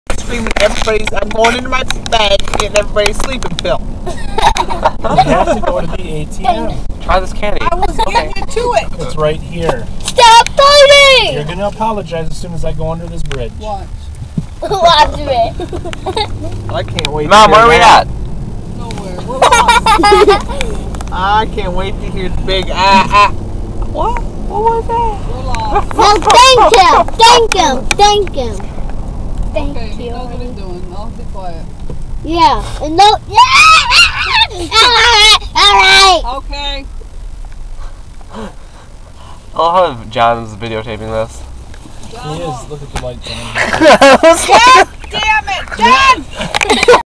Random Car Ride With Drake
Pretty much us lost trying to find the boat to Mackinac Island XD. One last thing. "GOD DAMMIT JON" XDDD